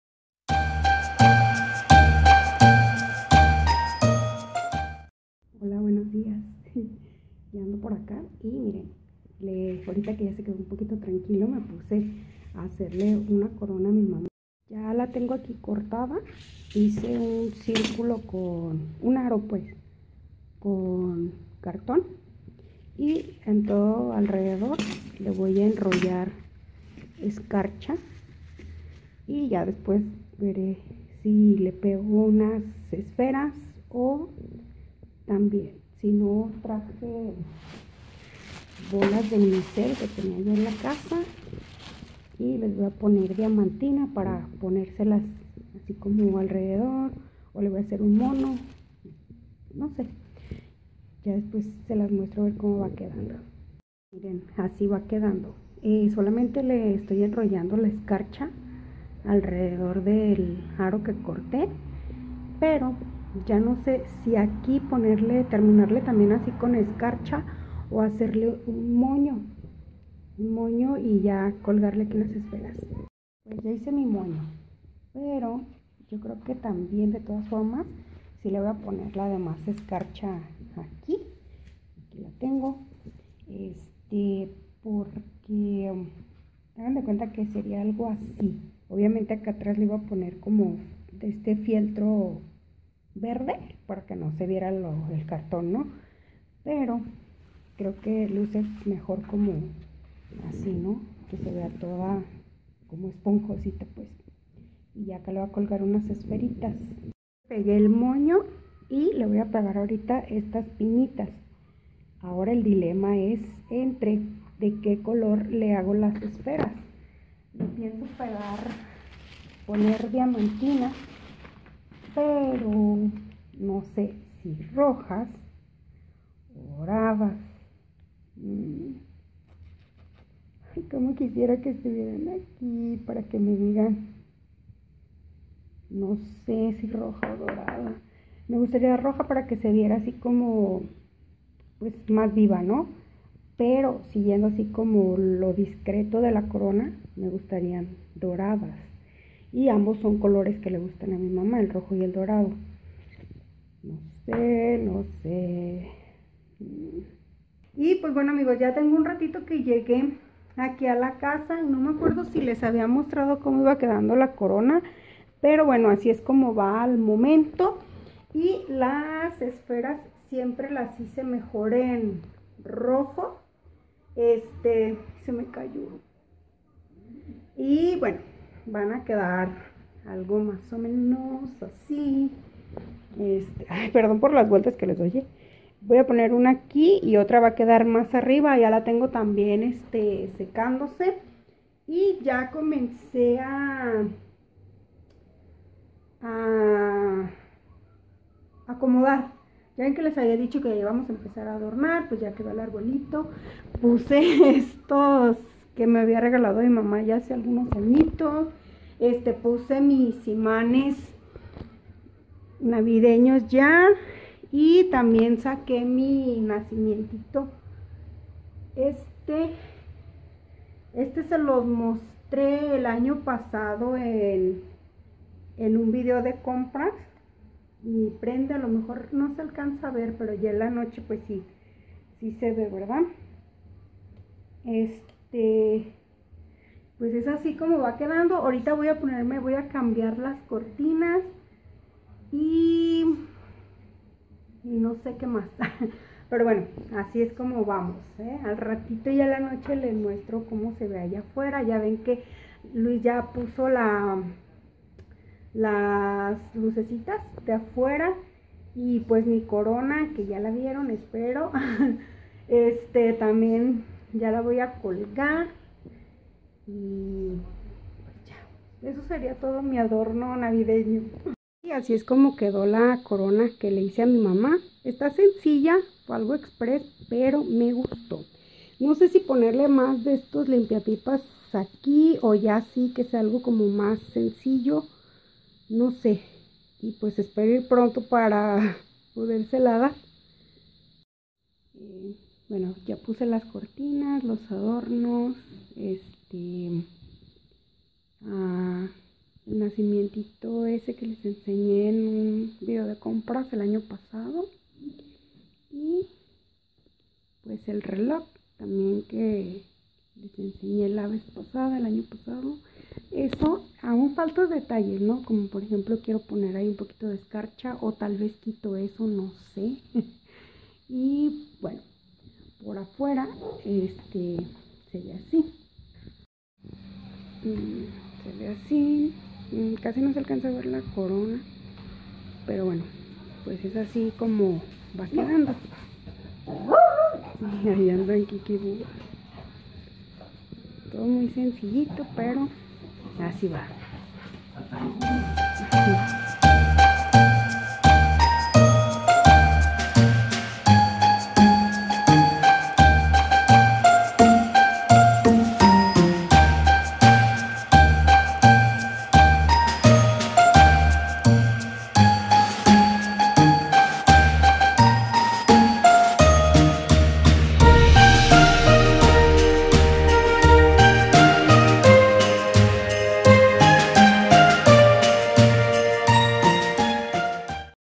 Vlog #192 Le hice una corona a mi mamá *VLOGMAS* | DETODOSVLOG
Hola amigos! Este vlog es algo super cortito, es lo mas relevante que hice el dia de ayer, lo mas productivo que hice despues de tranajr fue hacerle una corona navideña para mi mamá, espero que les guste y les sirva la idea si aun no tienen corona para su casa

COMPARTE
COMENTA

°°AQUI TE DEJO UNA PEQUEÑA LISTA CON MAS VLOGS°°

laura.mendoza.5011

muchas gracias por haberme acompañado en este video